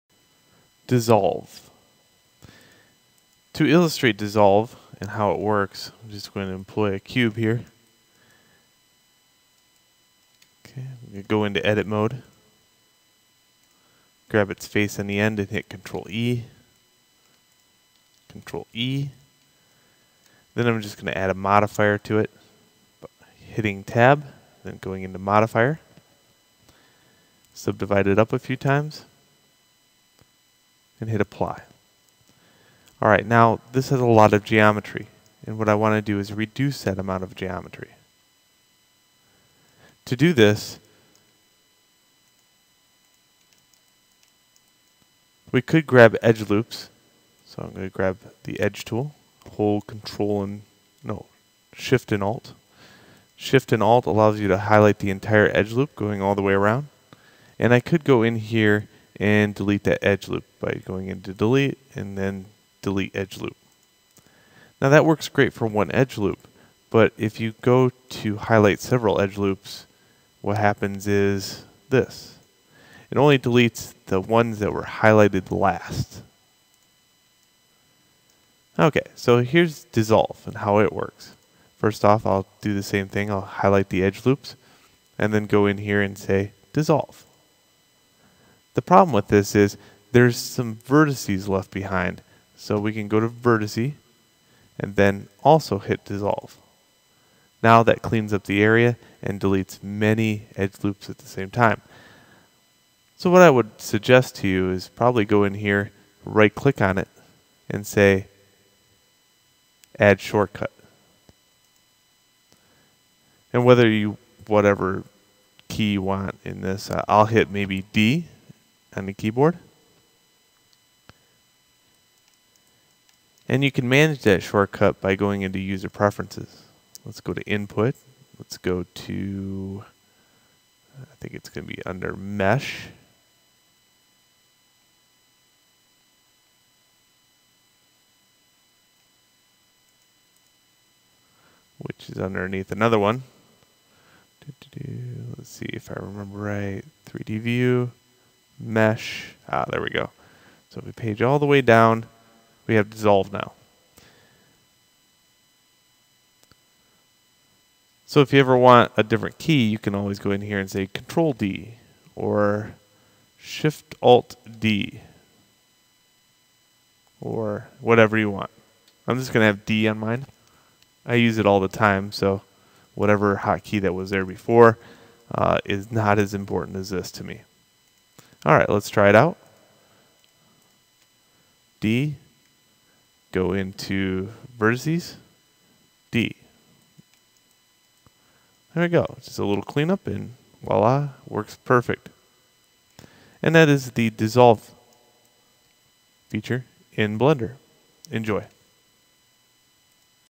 Blender2.63 Modeling tools_(Dissolve Command).mp4
Some updates to my modeling I series in blender now that 2.63 is released